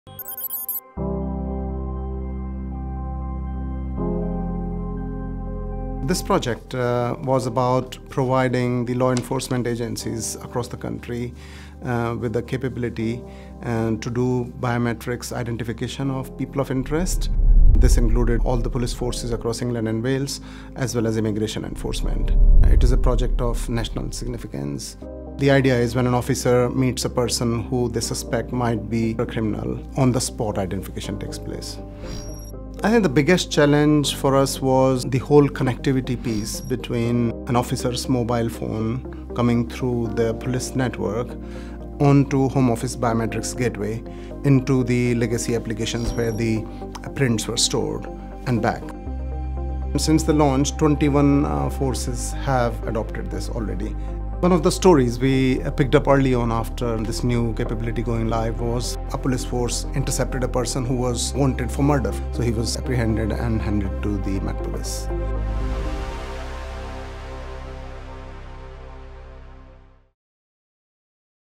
Home Office Biometrics Programme
The Home Office’s biometric technology is enabling police to fingerprint suspects on the spot.